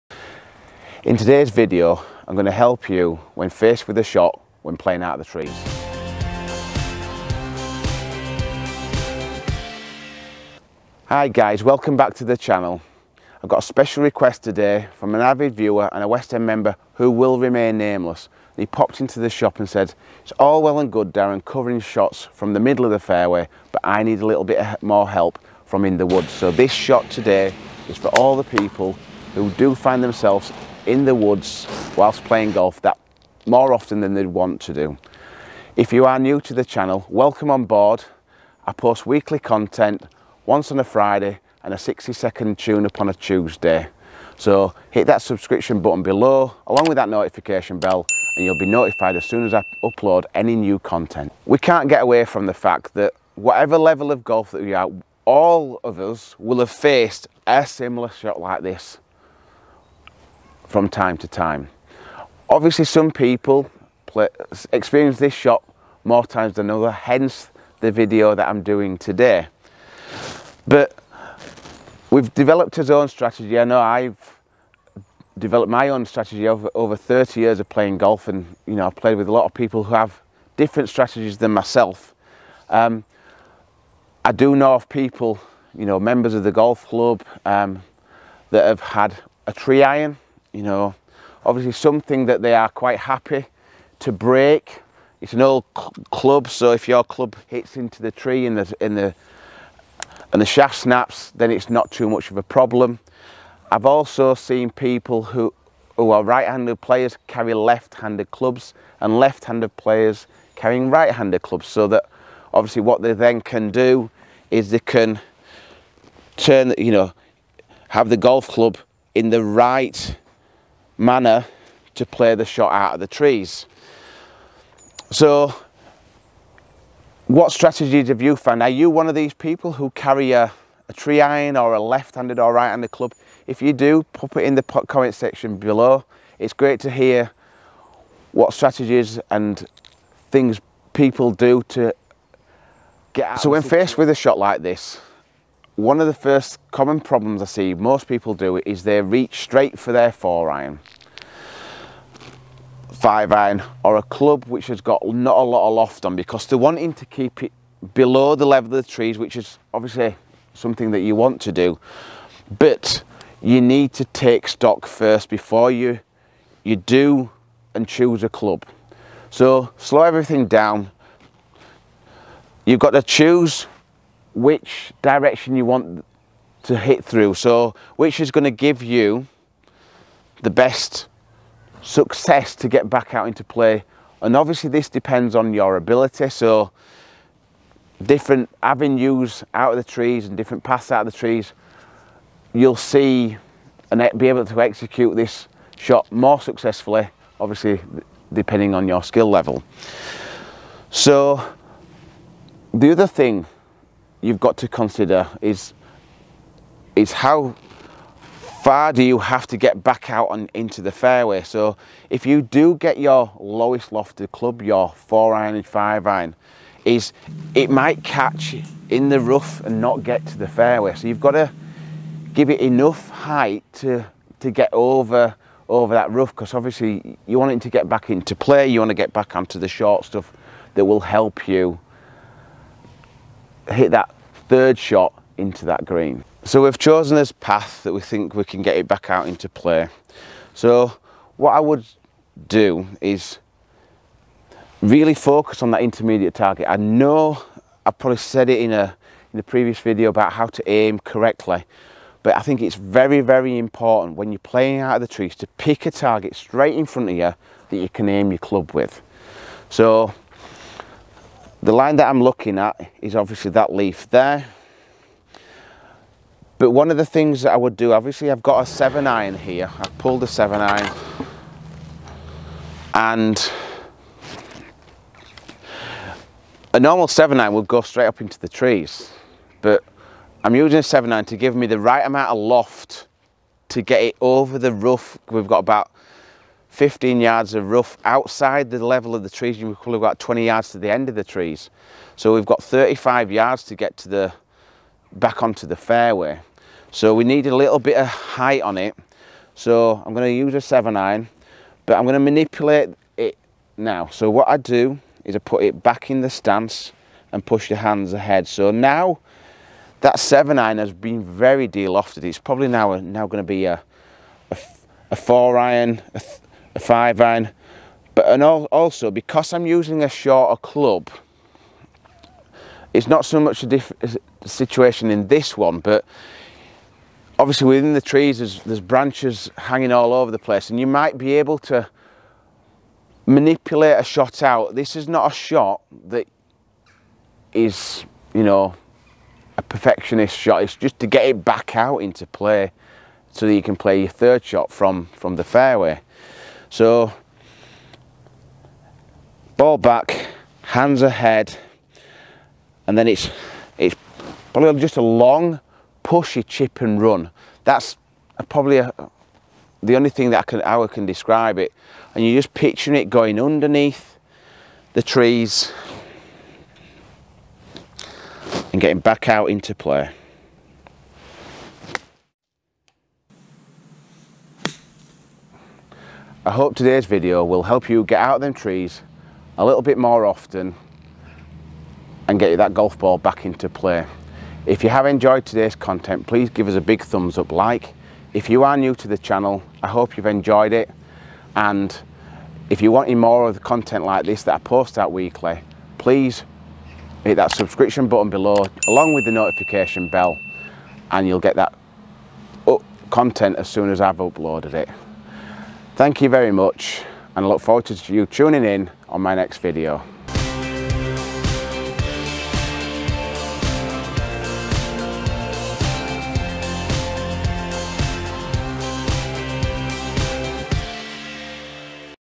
How to Hitting a Low Punch Under Trees - The Right Way
When you finds your ball in the trees what is the best strategy to successfully get your ball back into play. What things you should consider and how to hit the low punch out of the trees and get your ball back into play when you are in the trees.

Darren Arber will give you some on course strategy help so that you can get the ball back into play in with the least amount of stress.